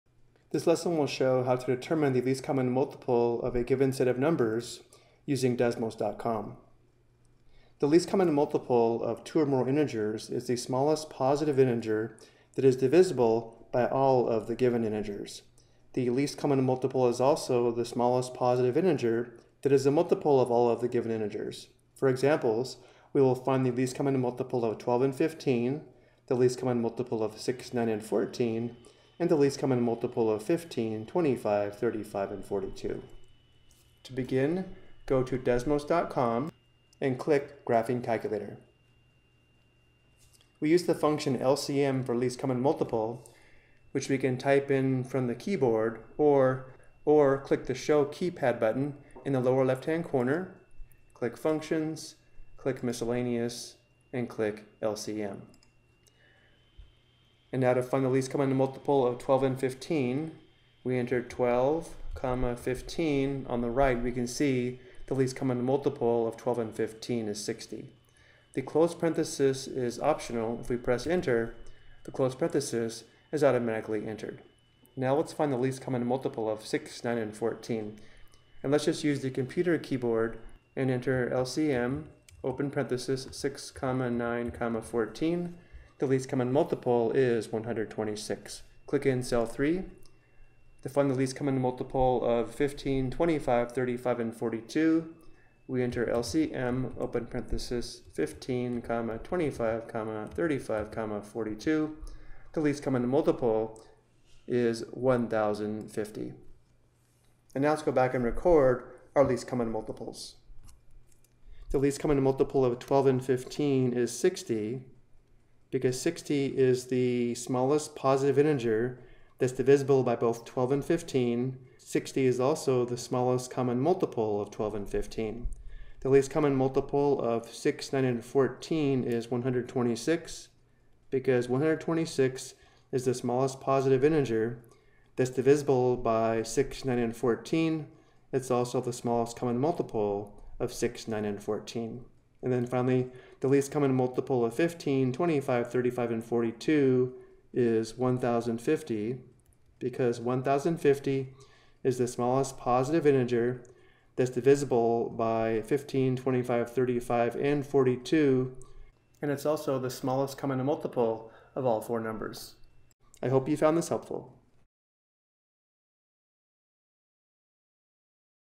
Least Common Multiple Using Desmos
This video explains how to find the least common multiple of a set of numbers using demsos.com